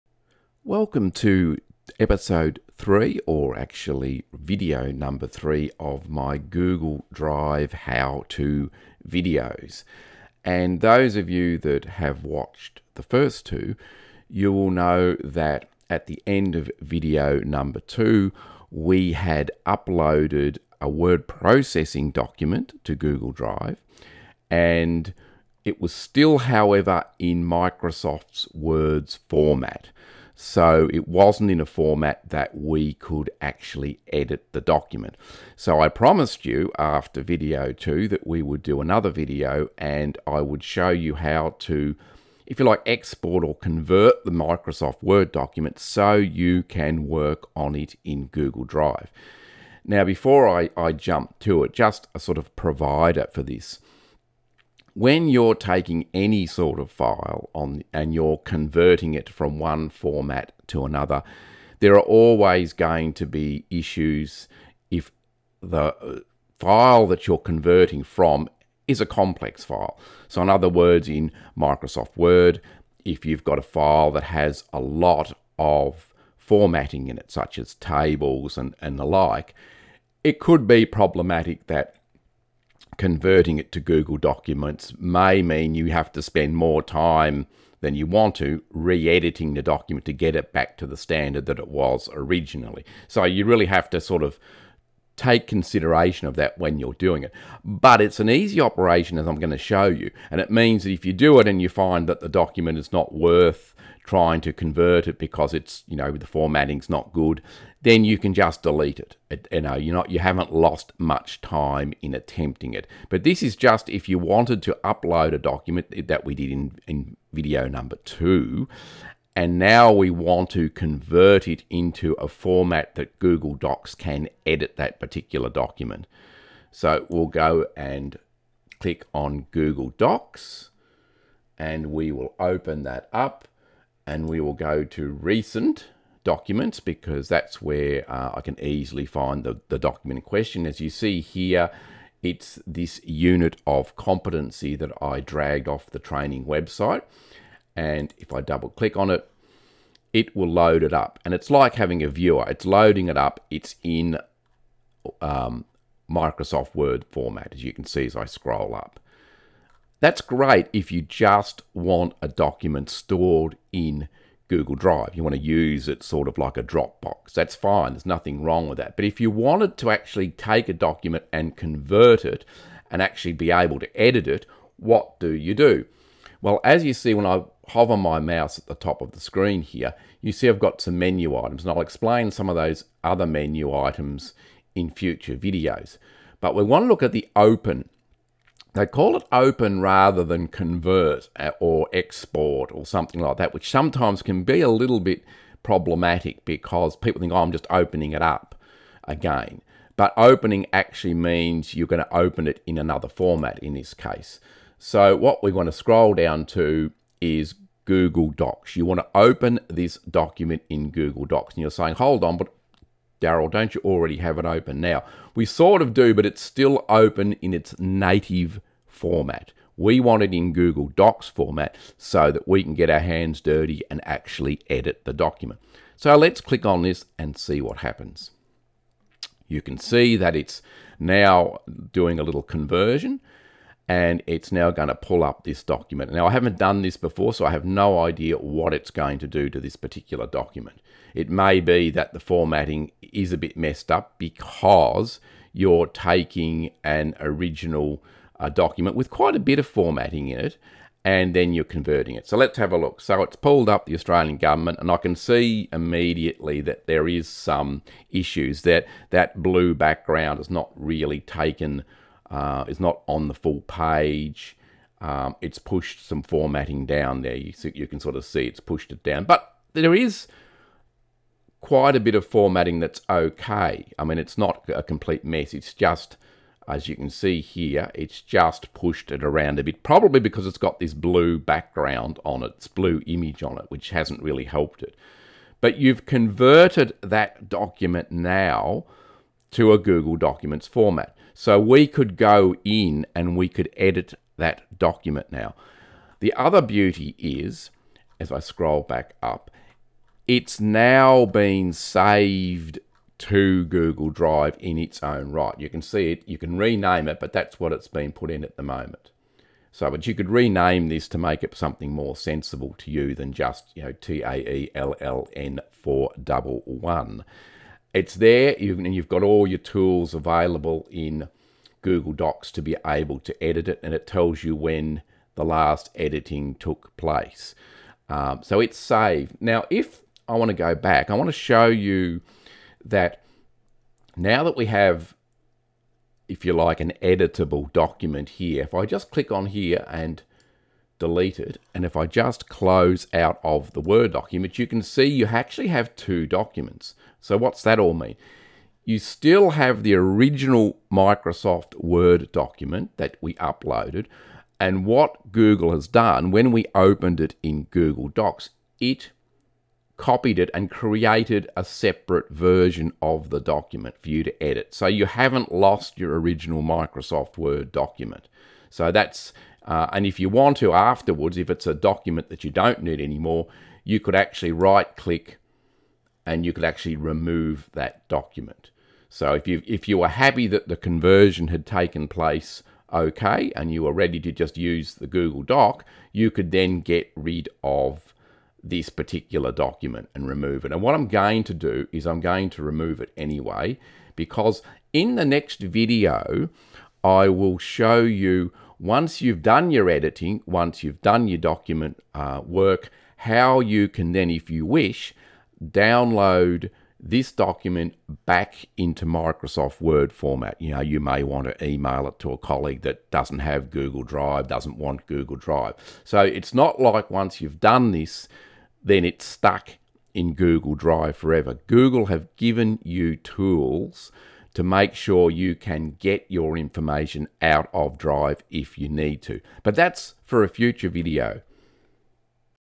How to Convert a Microsoft Word Document to Google Docs format
In this video I show you how to convert that uploaded Microsoft Word document to Google Drive format ready to be edited.